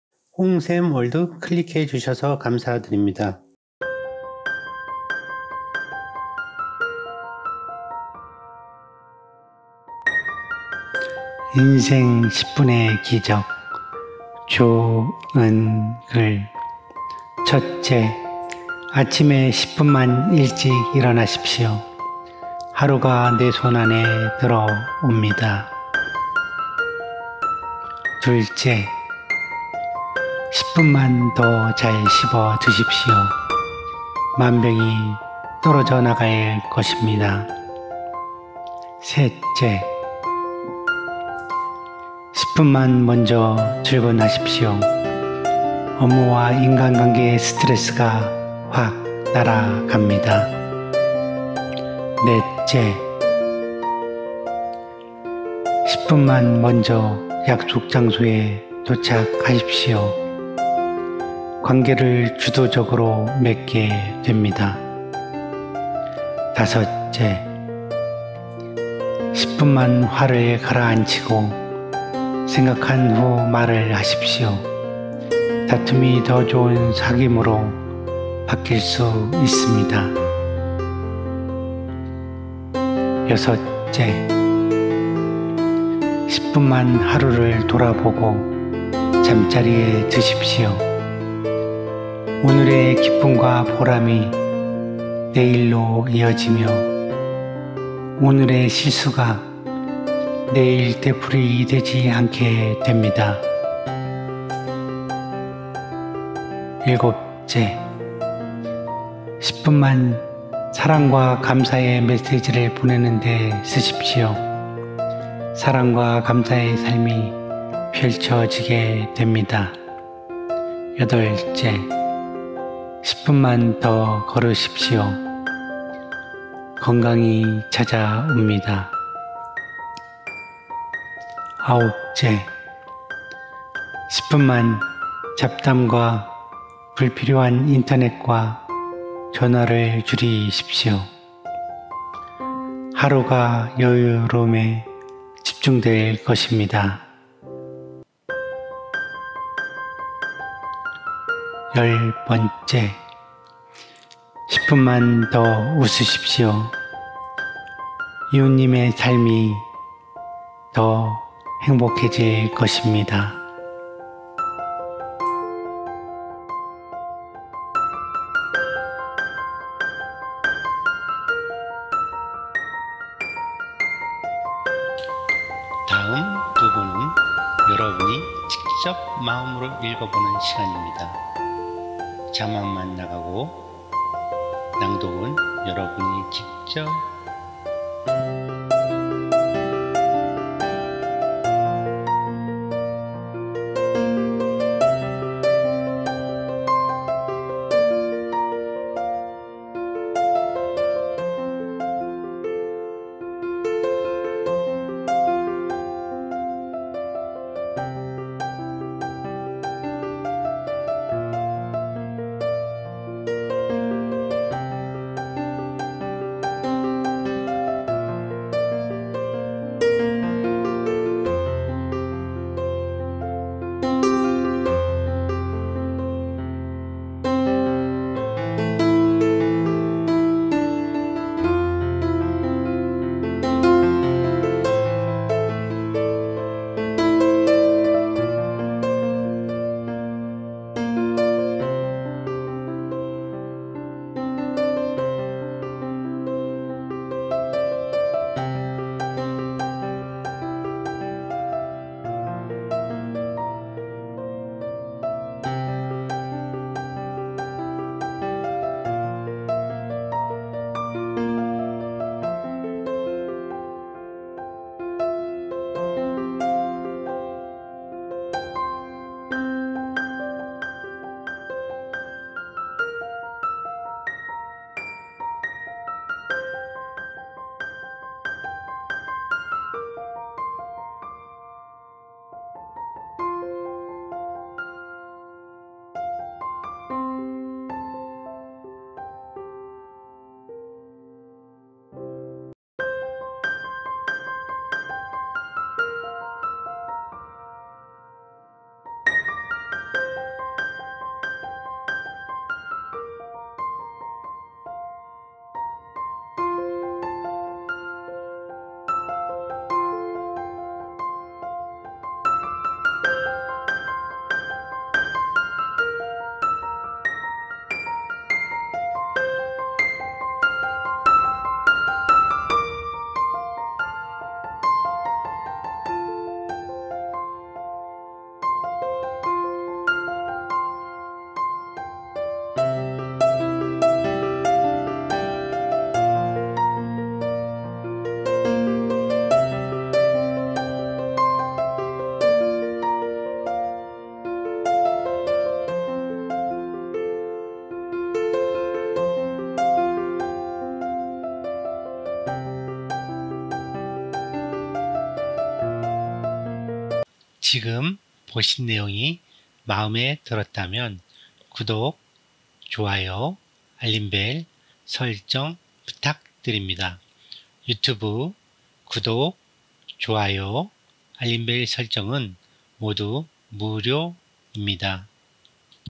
좋은글, + 인생 10분의 기적 / 좋은글, 10분만 더 웃으면 삶이 더 행복해질 것입니다.
좋은글,
인생 10분의 기적 / 좋은글,
10분만 더 웃으면 삶이 더 행복해질 것입니다.

첫째
아침에 10분만 일찍 일어나십시오.
하루가 내 손 안에 들어옵니다.

둘째
10분만 더 잘 씹어드십시오.
만병이 떨어져나갈 것입니다.

셋째
10분만 먼저 출근하십시오.
업무와 인간관계의 스트레스가
확 날아갑니다.

넷째
10분만 먼저 약속 장소에 도착하십시오.
관계를 주도적으로 맺게 됩니다.

다섯째
10분만 화를 가라앉히고
생각한 후 말을 하십시오.
다툼이 더 좋은 사귐으로 바뀔 수 있습니다.

여섯째
10분만 하루를 돌아보고
잠자리에 드십시오.
오늘의 기쁨과 보람이 내일로 이어지며
오늘의 실수가 내일 되풀이되지 않게 됩니다.

일곱째
10분만 사랑과 감사의
메시지를 보내는 데 쓰십시오.
사랑과 감사의 삶이 펼쳐지게 됩니다.

여덟째
10분만 더 걸으십시오.
건강이 찾아옵니다.

아홉째
10분만 잡담과 불필요한 인터넷과
전화를 줄이십시오.
하루가 여유로운 중에 집중될 것입니다.

열번째
10분만 더 웃으십시오.
이웃님의 삶이 더 행복해질 것입니다.